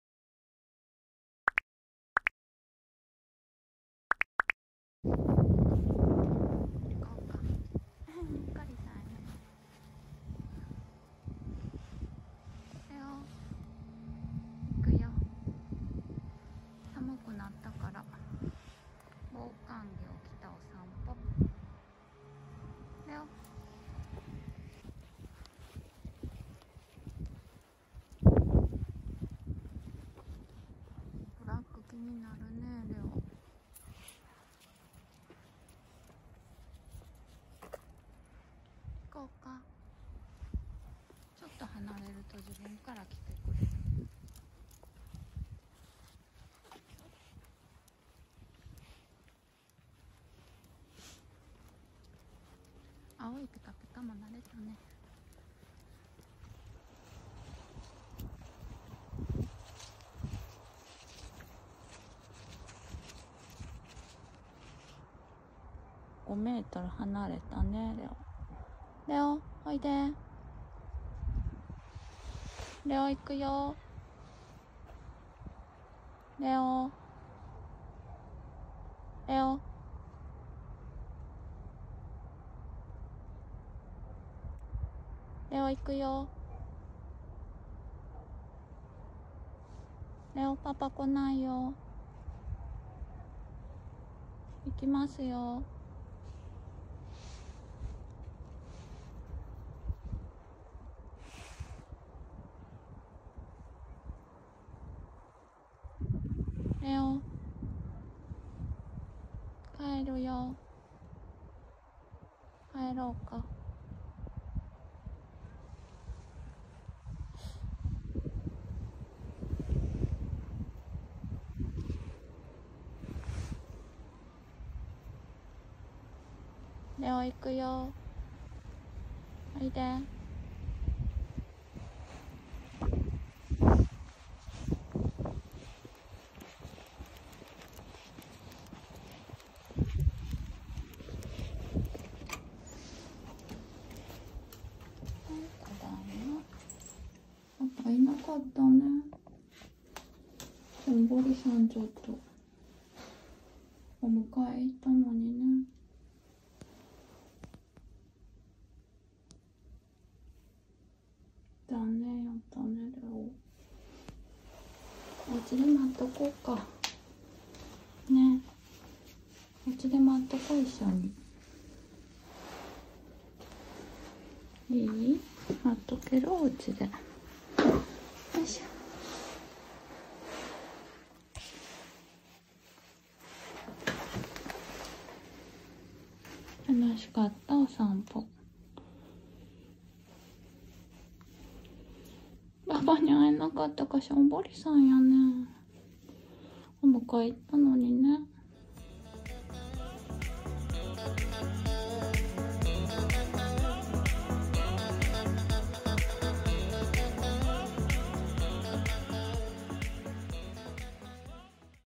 【ホワイトシェパード】れおの毎日１３８ 夕方の散歩で帰ってくると思ったパパが帰ってこなかった。。。
視聴ありがとうございます。ホワイトシェパードのれおです。

夕方の散歩は、いつもお迎えもします。
今回は時間が違うかったみたいです。。

■れおのプロフィール

　犬種：ホワイトシェパード
　誕生日：2019年3月27日生まれ
　性別：男の子
　体重：44kg
　性格：おっとり控えめ